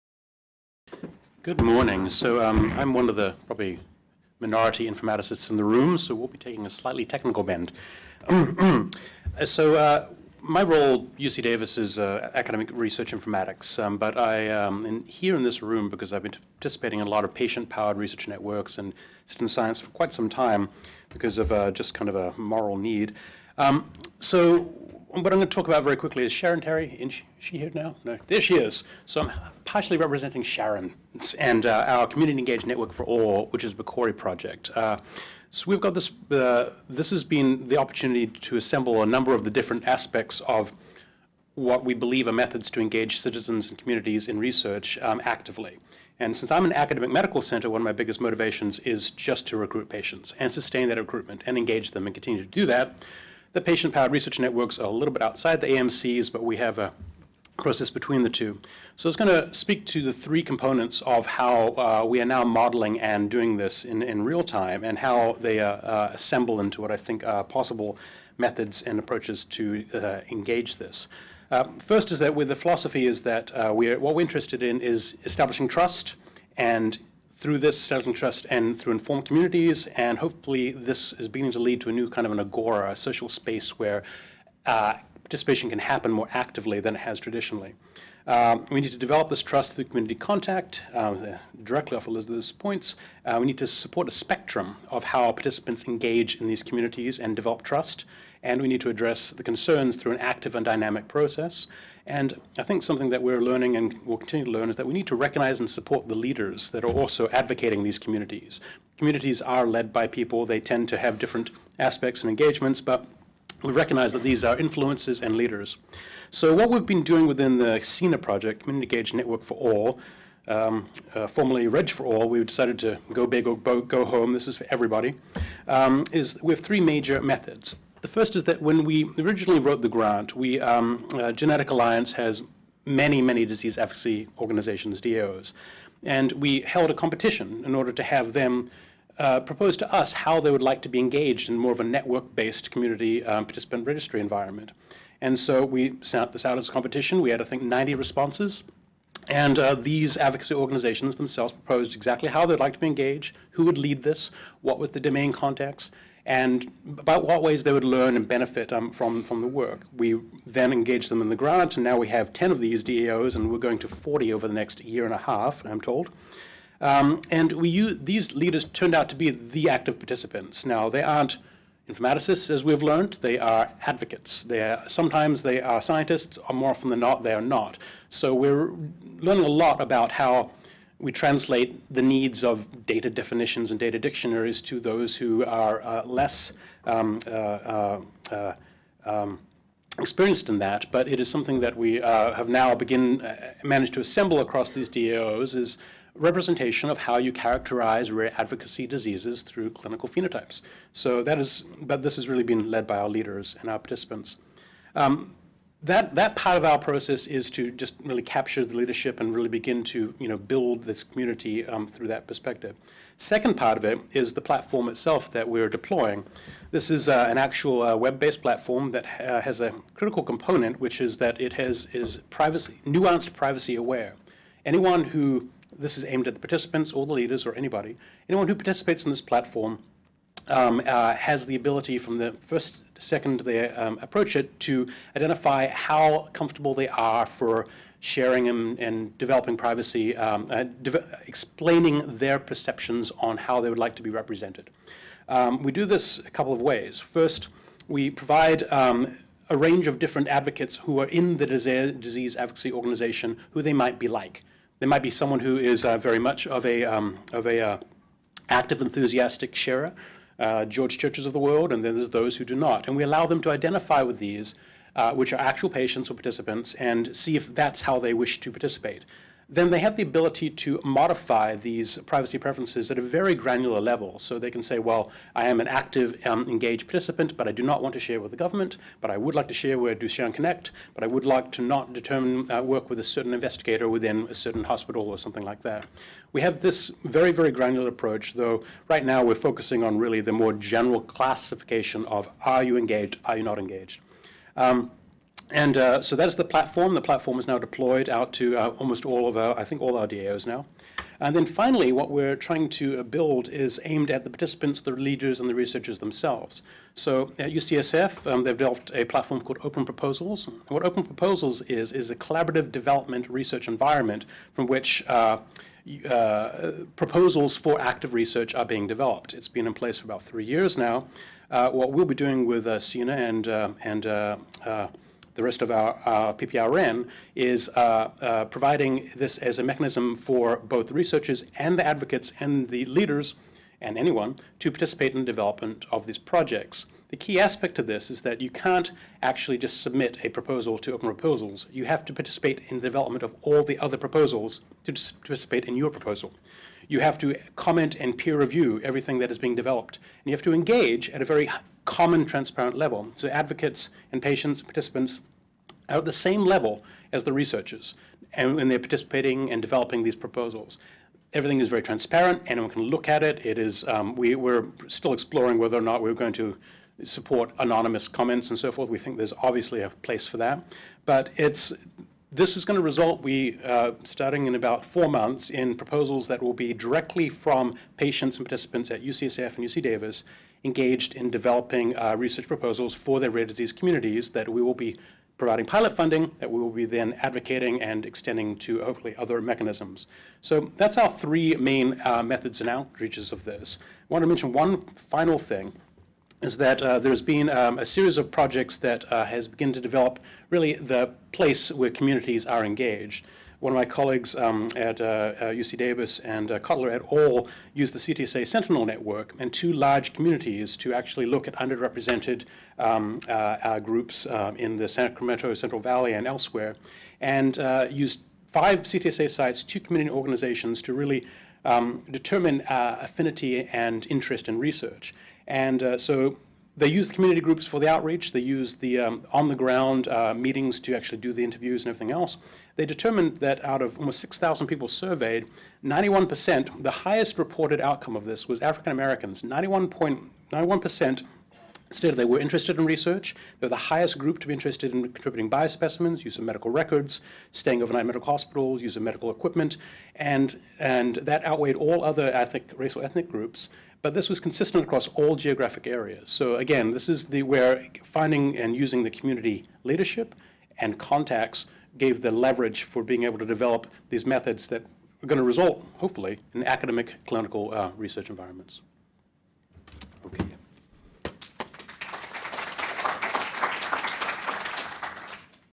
Session 2: Building the Relationship: Citizen and Community Engagement ... - Nick Anderson
January 13-14, 2015 - Trans-NIH Workshop to Explore the Ethical, Legal and Social Implications (ELSI) of Citizen Science